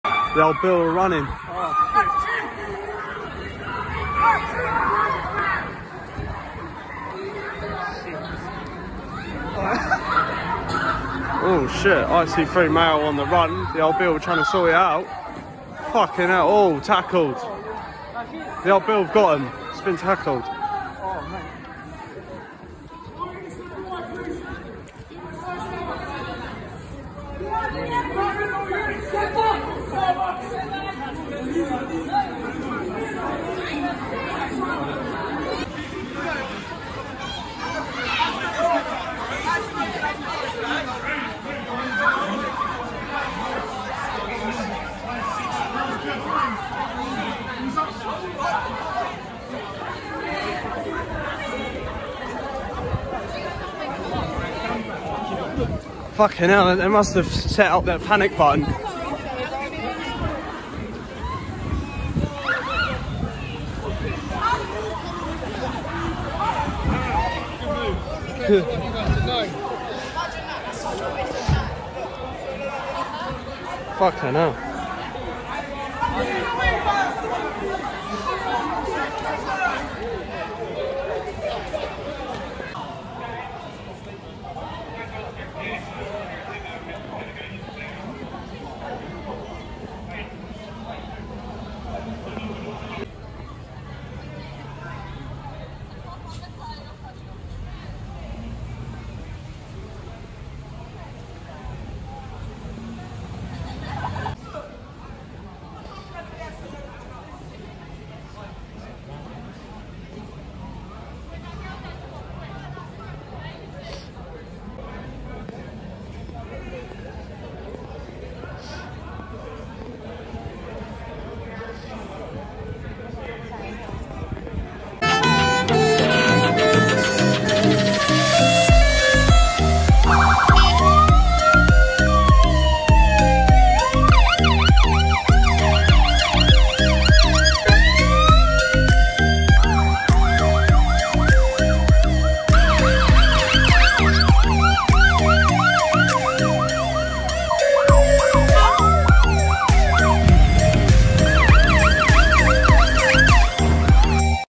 *URGENT ASSISTANCE NEEDED* Metropolitan Police Footchase and tackle caught on camera!
Displayed here is a police foot chase that leads to multiple officers running after a school kid a officer was also seen throwing their police hat and missing following that the suspect was tackled by a officer then helped by others it was then when the urgent call came through due to lord of school kids gathering the officers then lots of officers were made to respond quickly and provide assistance this backup contained RTPC, ERPT, VSU.